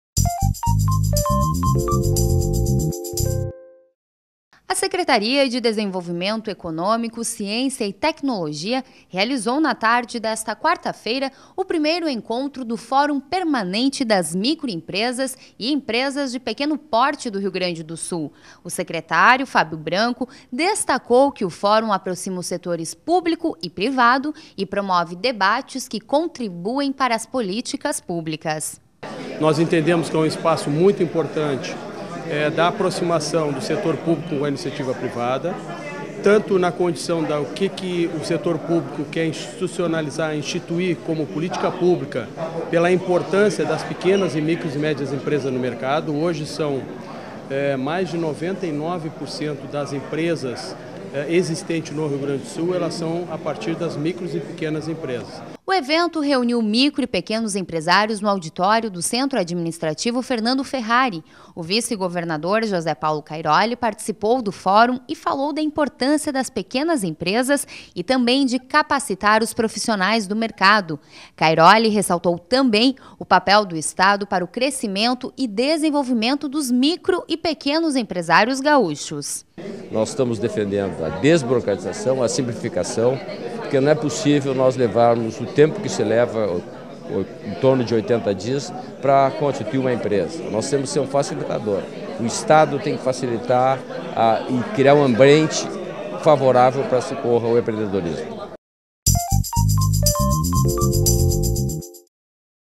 Fórum permanente de pequenas e microempresas retoma atividades
O Fórum Estadual Permanente de Microempresas e Empresas de Pequeno Porte, um instrumento para reunir integrantes do segmento do setor privado com o público, visando ao debate e elaboração de políticas públicas que fomente o empreendorismo, foi retomado no Rio Grande do Sul. Nesta quarta-feira (12), em evento no Centro Administrativo Fernando Ferrari (Caff), em Porto Alegre, o vice-governador José Paulo Cairoli e o secretário do Desenvolvimento, Ciência e Tecnologia, Fábio Branco, apresentaram a agenda executiva do Fopemepe/RS e participaram do termo de posse dos integrantes do fórum estadual.